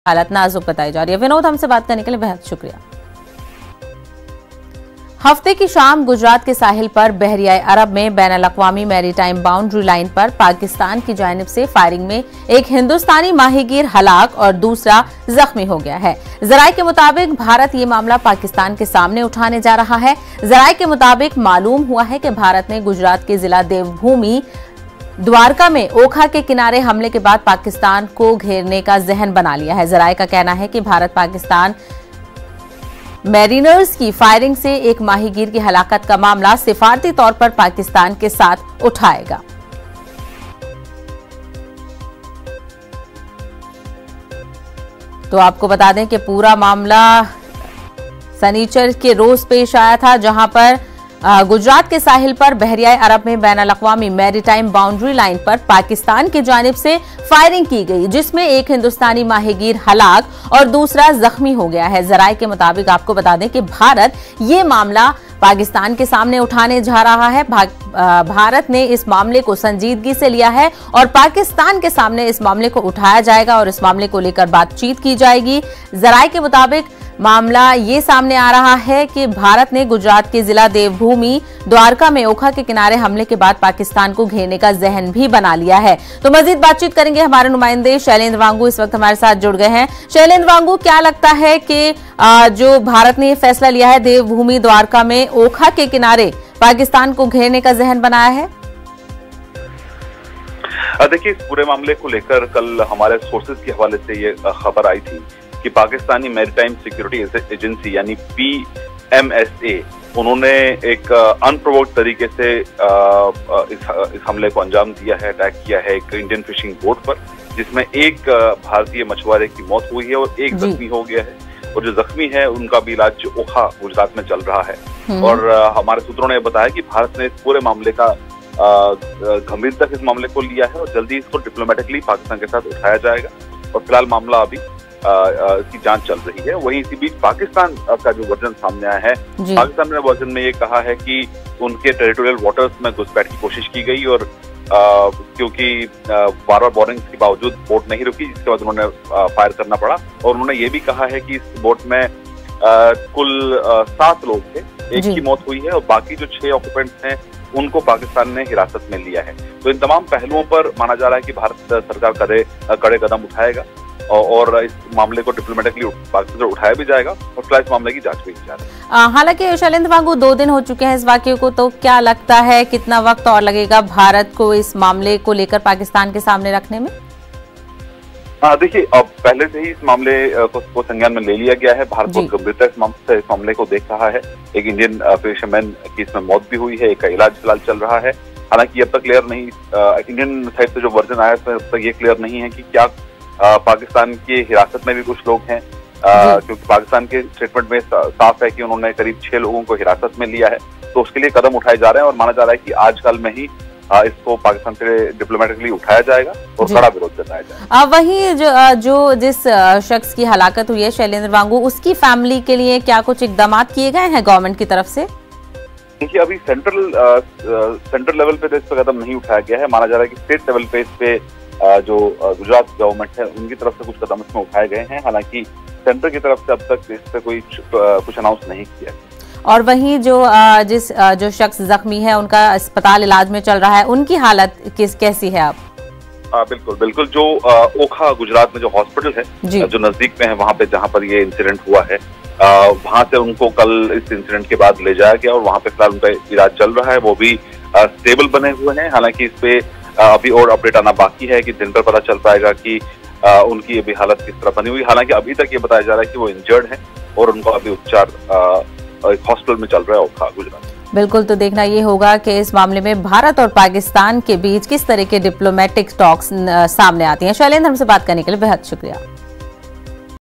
Gujarat News: Arabian Sea Mein Pakistan Ki Firing Se Ek Fisherman Ki Maut | News18 Urdu
An Indian fisherman died after the Pakistan Maritime Security Agency (PMSA) fired at his boat near the international maritime boundary line in the Arabian Sea off the Gujarat coast. One of the seven crew members aboard the boat sustained a minor injury in the incident and is being treated at a hospital in Gujarat's Okha.